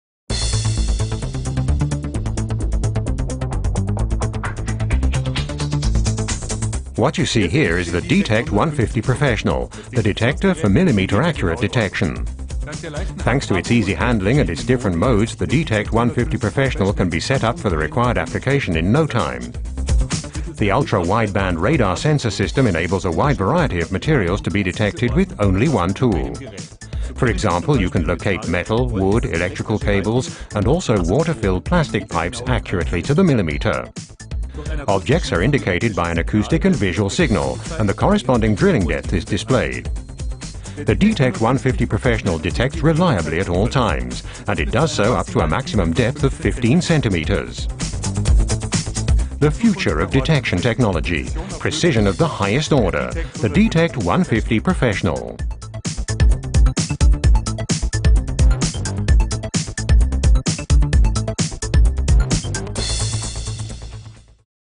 Bosch D-Tect 150 - Wall Scanner / Stud Finder UWB Radar
Bosch D-tect 150 is the best professional wall scanner used for achieving maximum precision and accuracy. The D-tect 150 has ultra wideband radar(UWB radar) sensor system which makes it possible for a millimeter-accurate detection of ferrous/non- ferrous metals, live cables, wooden beams and plastic pipes such as floor and wall heating pipes, providing precise detection of reinforcing steel in concrete up to 15 cm depth with digital display of material properties and maximum permitted drilling depth. Bosch wall scanner has a dust and splash protection which is IP 54. Live wires may not be detected in certain conditions such as in metal conduit or behind metalized or conductive surface. It consists of 7 detection modes which includes concrete, wet concrete, deep concrete, in-floor heating, drywall, metal and signal view.